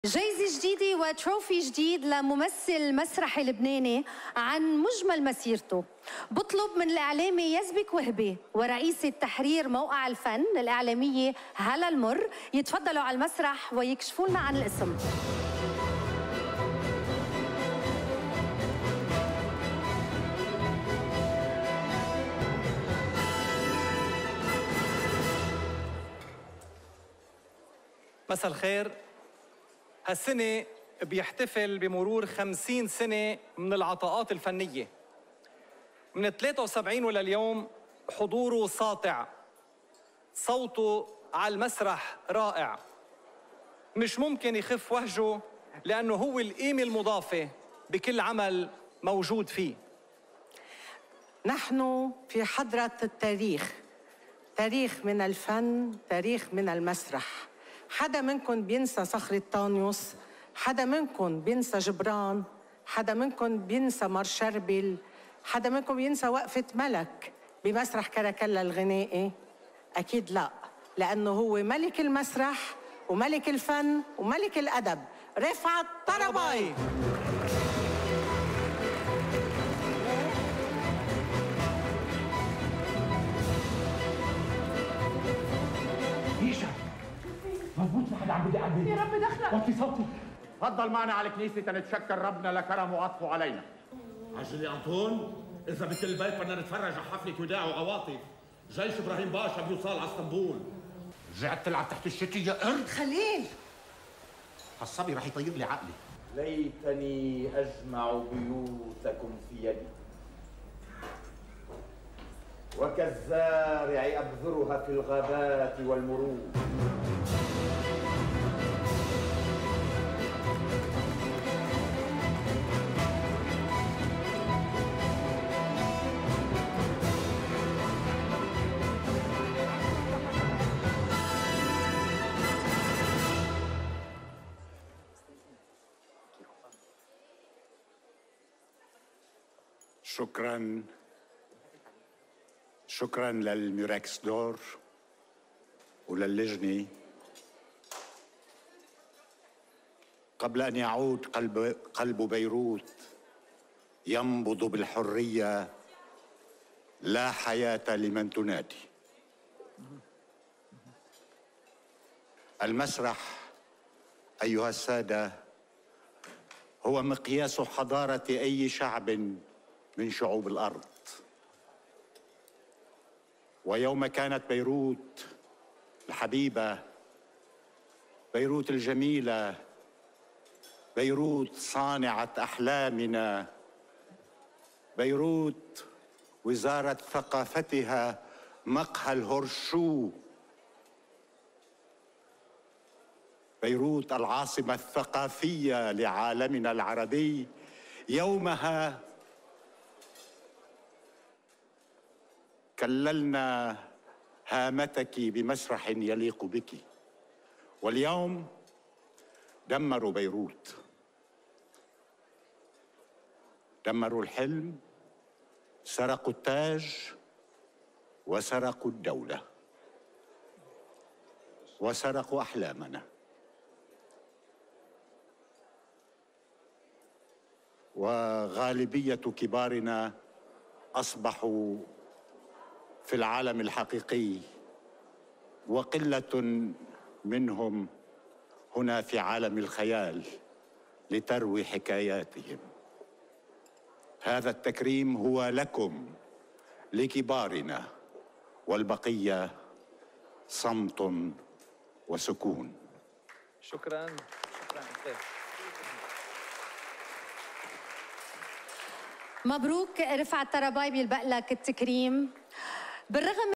Murex D'or 2023
موركس تكريمي لممثل مسرحي لبناني عن مجمل مسيرته

The Murex d'Or (also known as the Golden Murex award) is a Lebanese award created by Lebanese physicians, Zahi and Fadi Helou to recognize achievements in the domain of art in Lebanon, the Arab region and the world.